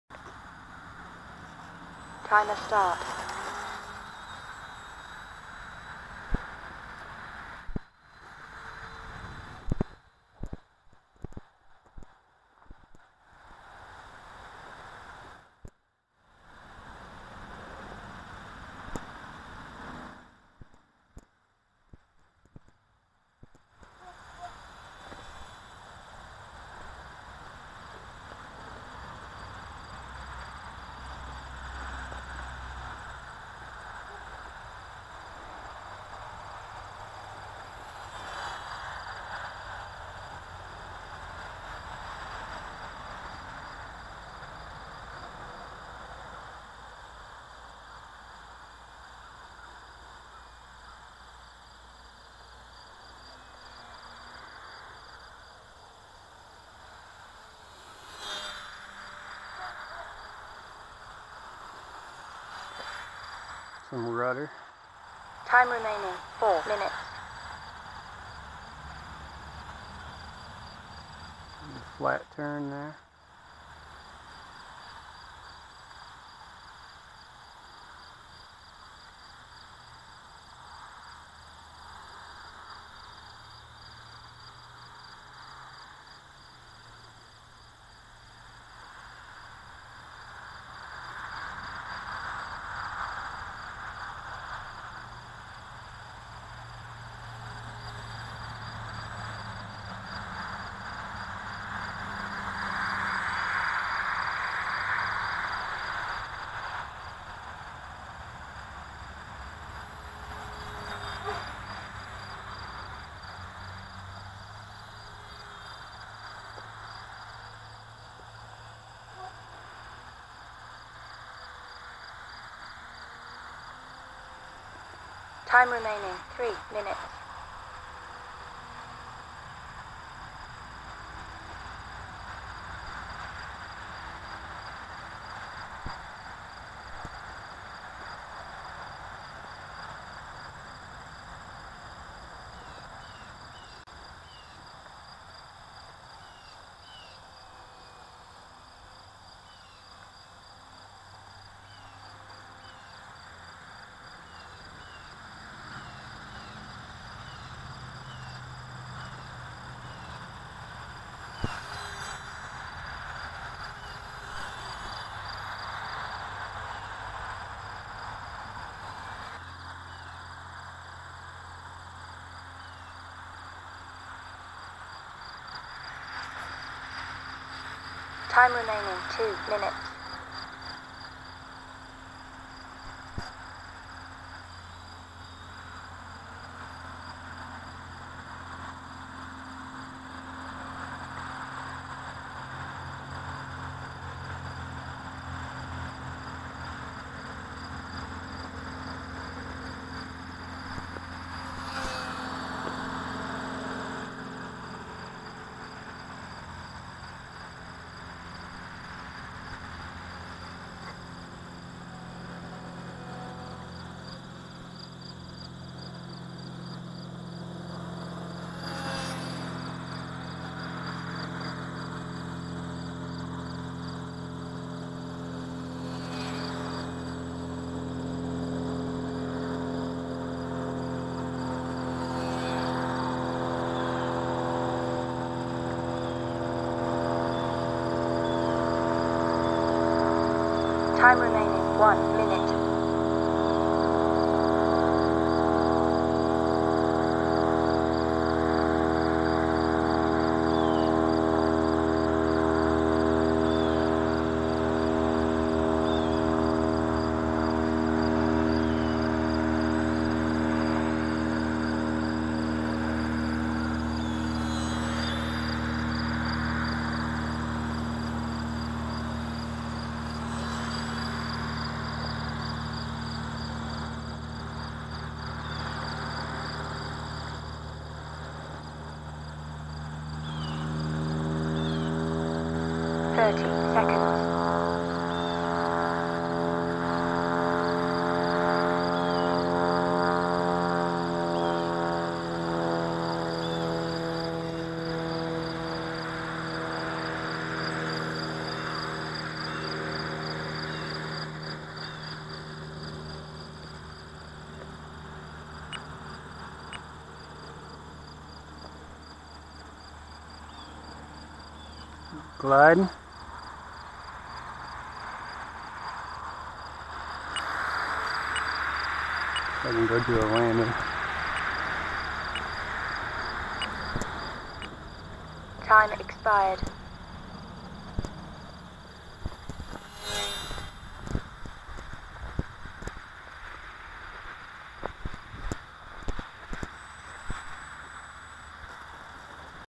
HobbyZone Sport Cub S - Sporty Flight
The wind died down and I had a nice little flight towards the end of the day.  This little plane is pretty nice, it is great for beginners, no doubt, but it is also pretty sporty.